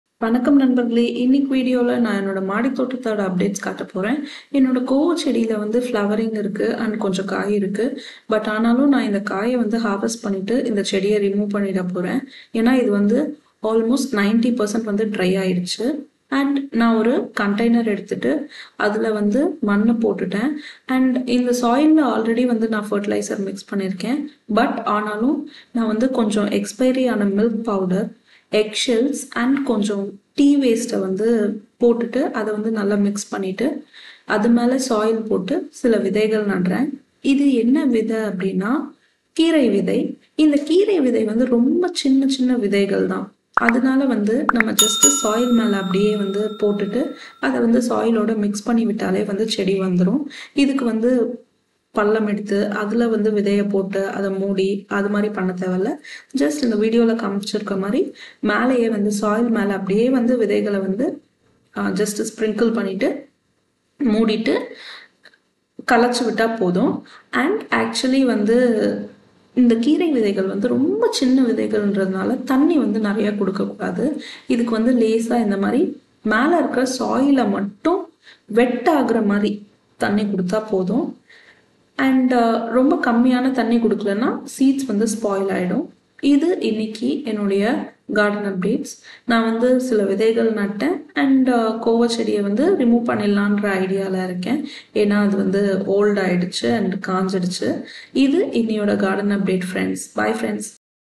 PLANTING NEW SEEDS OF KEERAI
In this video we are going to see how to grow plants without chemicals.

Planting keerai.